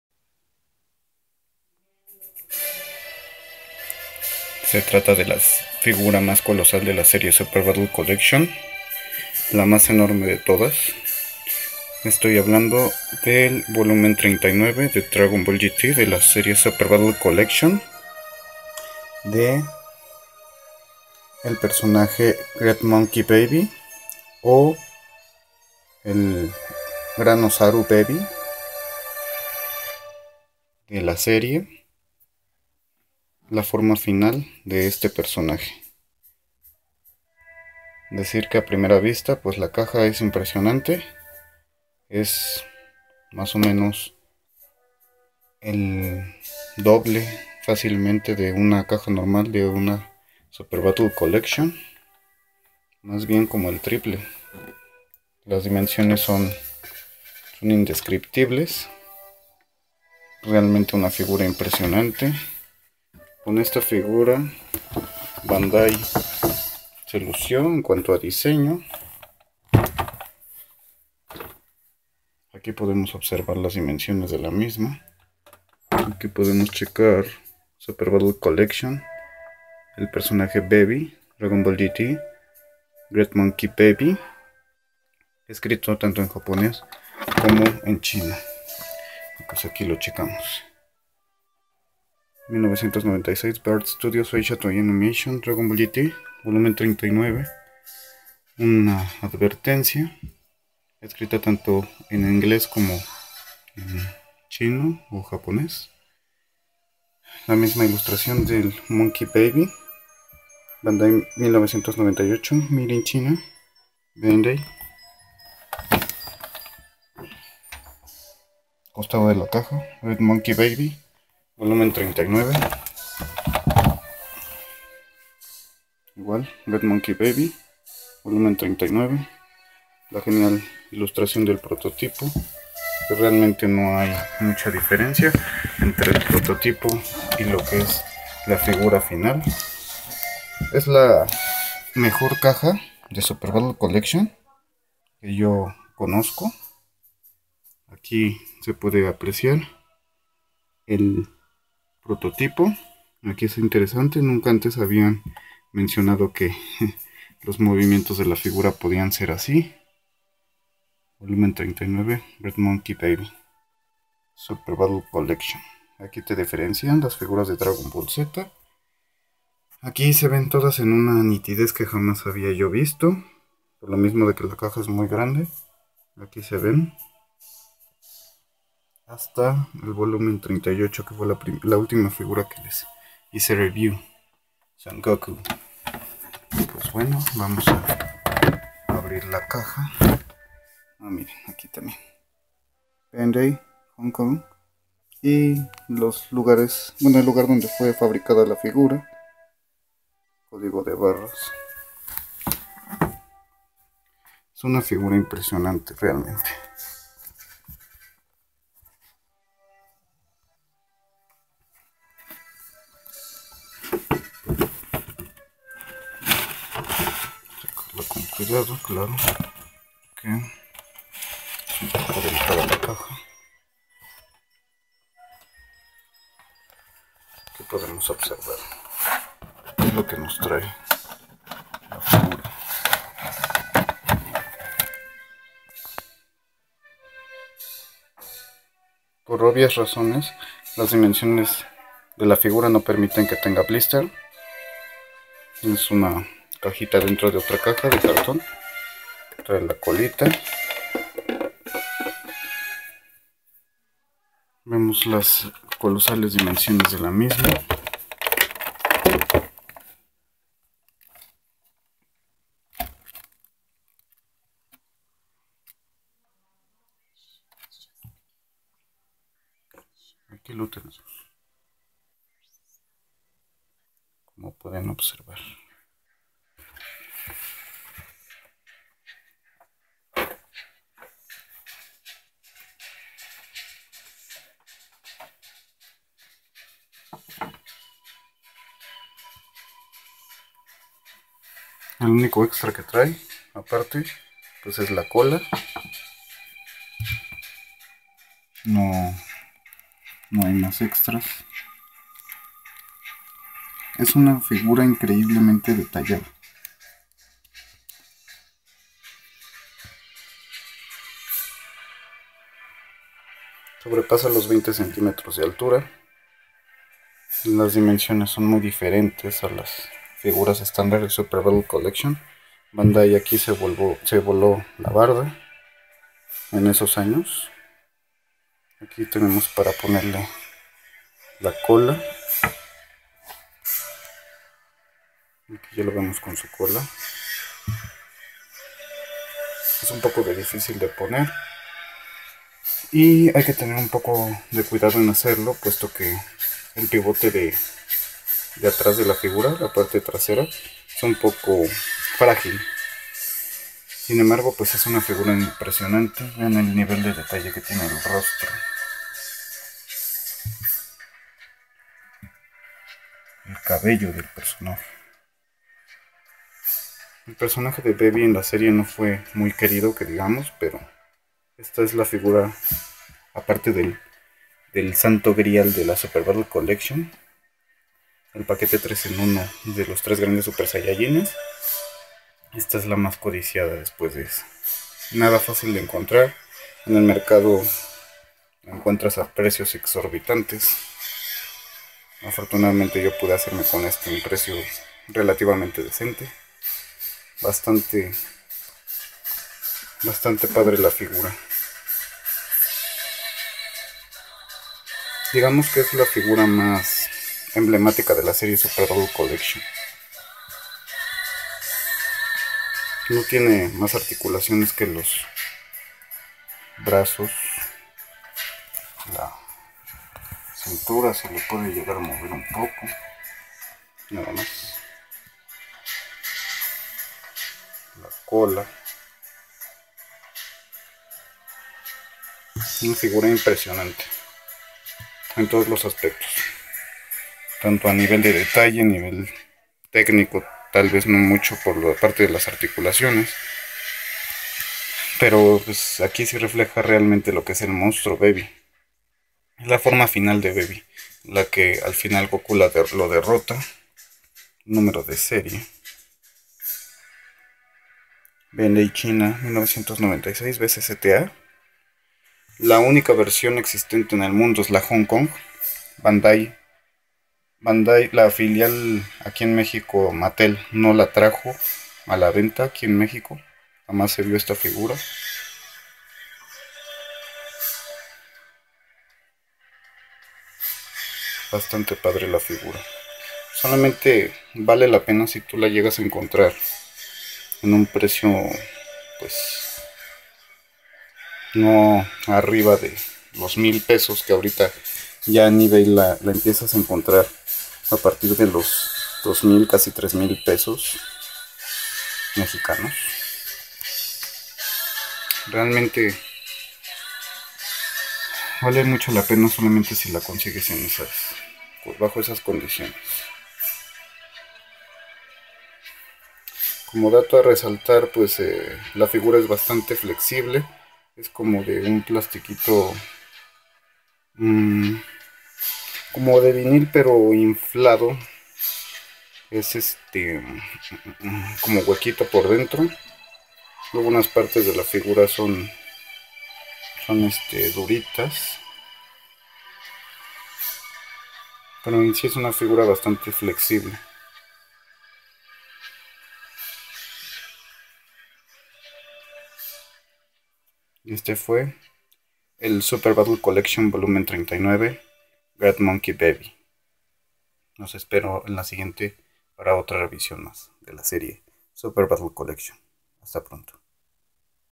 Dragon Ball GT Super Battle Collection Vol. 39 Great Monkey Baby Bandai HK 1998.
Revision de la colosal figura de Dragon Ball GT Super Battle Collection Vol. 39 Great Monkey Baby Bandai HK 1998. La unica figura de DBGT que no fue editada en Mexico por Mattel en el 2000.